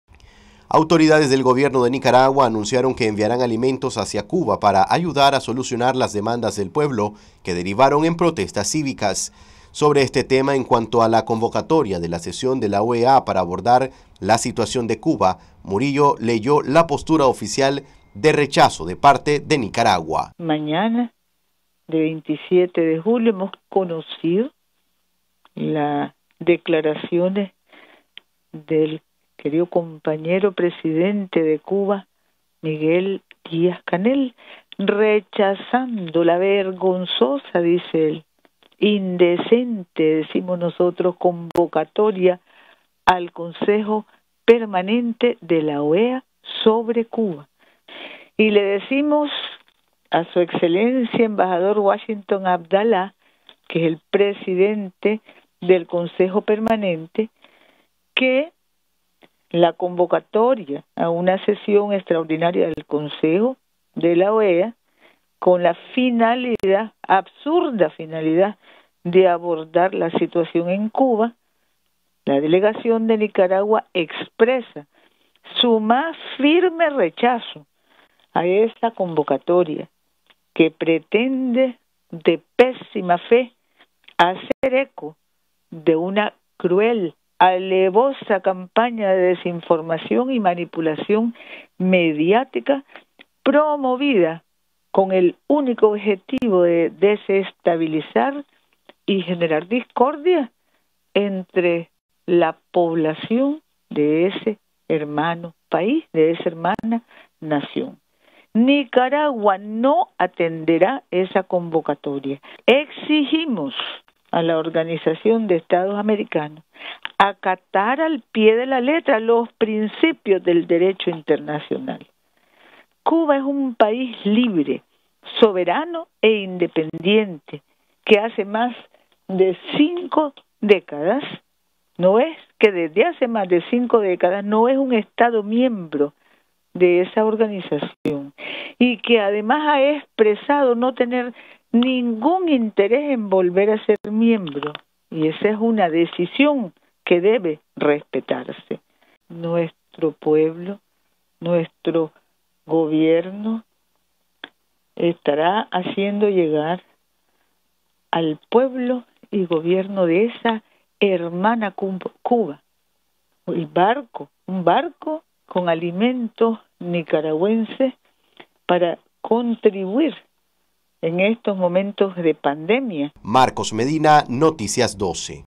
Gobierno de Nicaragua anuncia el envío de barco con alimentos para Cuba
Noticias12Nicaragua Gobierno de Nicaragua anuncia el envío de barco con alimentos para Cuba.